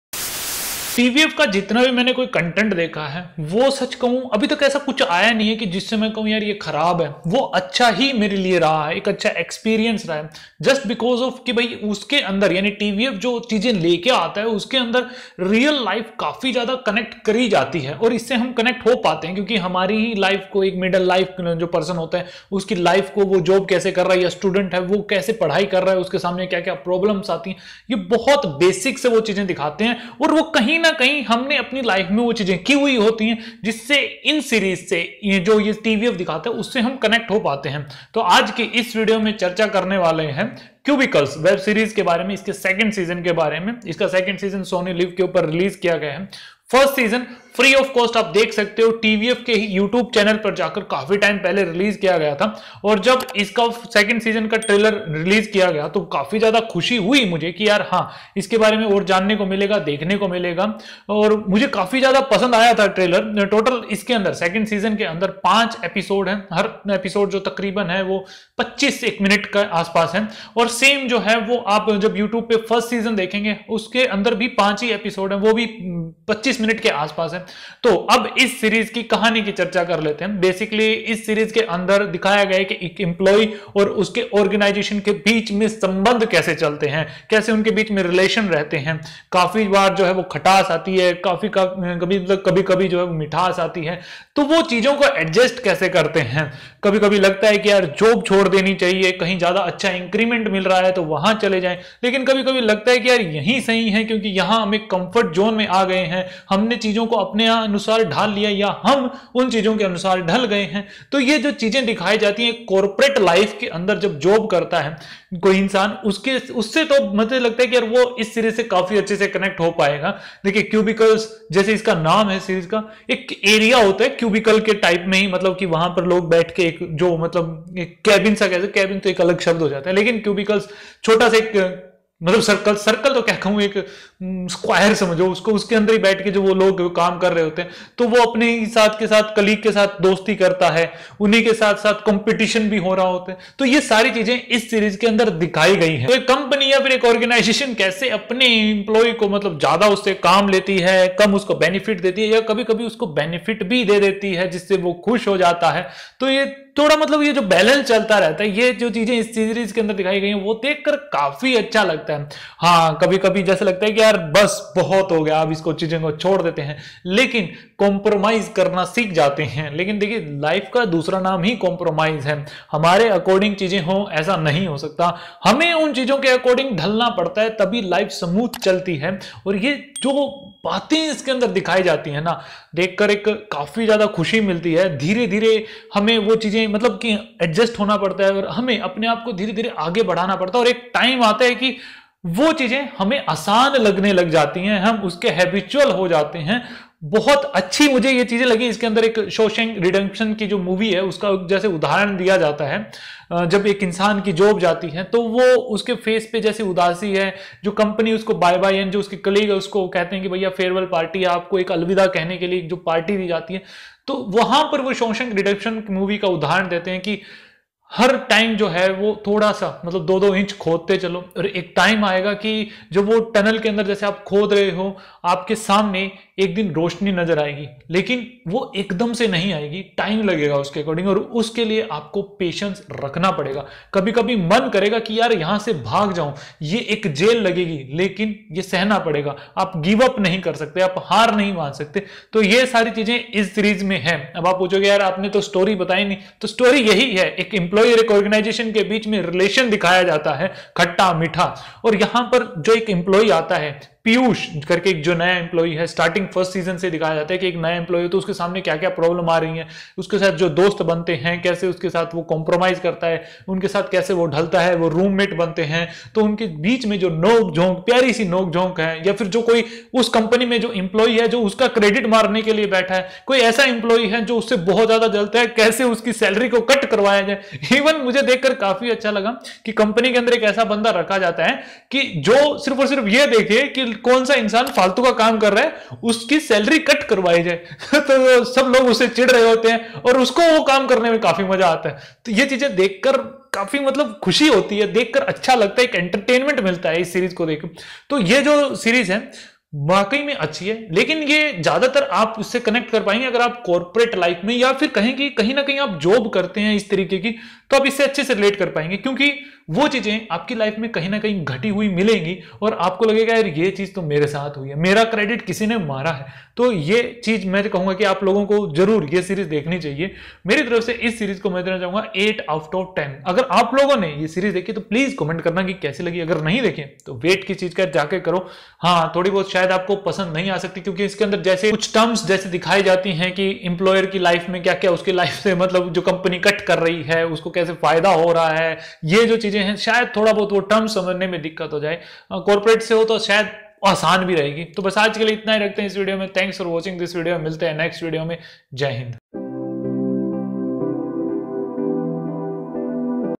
Cubicles Web Series Season 2 Review @SonyLIV | Abhishek Chauhan, Nidhi Bisht, Badri Chavan
This world is actually run by people sitting and working in their cubicles. This is a story of Piyush Prajapati, a 22 year old, fresh out of college and recruited like most of his batch-mates into an Indian IT company. From the time he gets his first salary, working weekends, work-life balance, a little bit of heart to all kinds of highs and lows, the show follows many firsts and chronicles the life of Piyush, a typical Indian first jobber and the people around him.

cubicles season 2 full download,